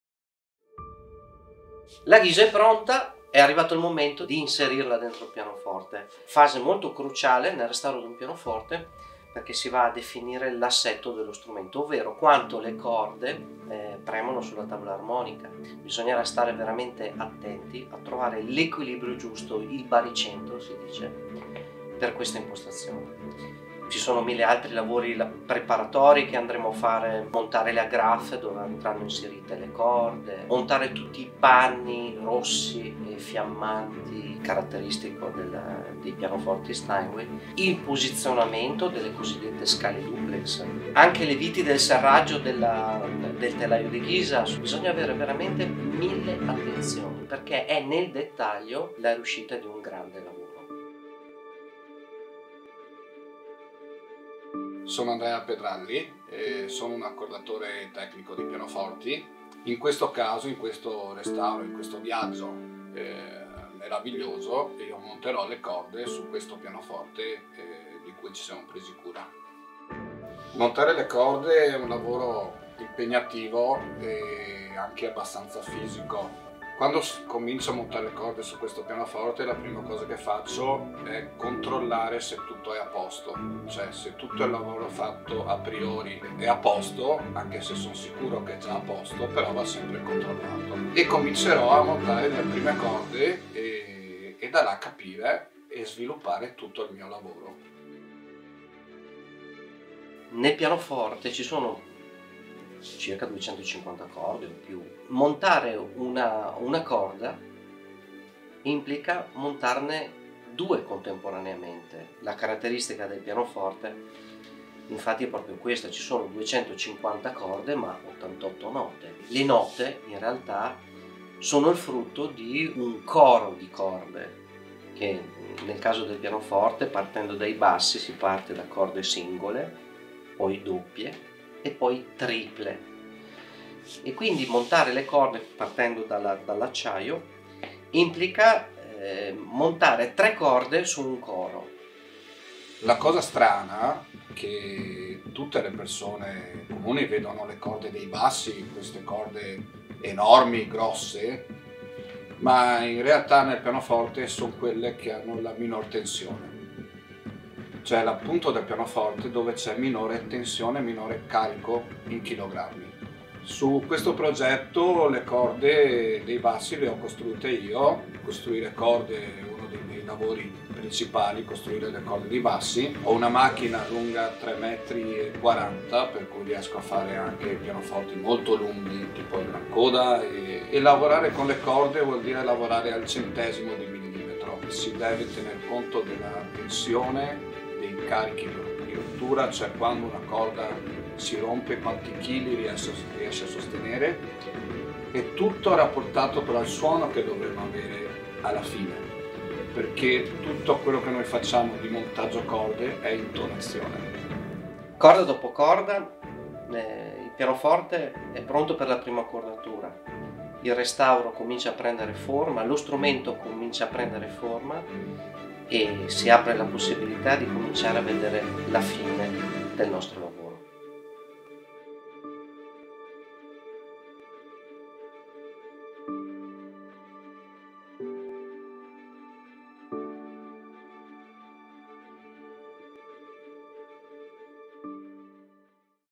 BRING A PIANO BACK TO LIFE - EP 13
Meet master technician Andrea Pedralli, who built and set the strings on this Steinway model B piano. The adventures of a piano and its pianist are nearing the end... To find out more, or to participate in the project, please / Nel tredicesimo episodio delle avventure di un pianoforte e del suo pianista, conosciamo Andrea Pedralli, il maestro tecnico che ha costruito e montato le corde su questo Steinway modello B. Il lavoro comincia ad avvicinarsi alla fine... Per saperne di più, e per partecipare attivamente al progetto, prego visitare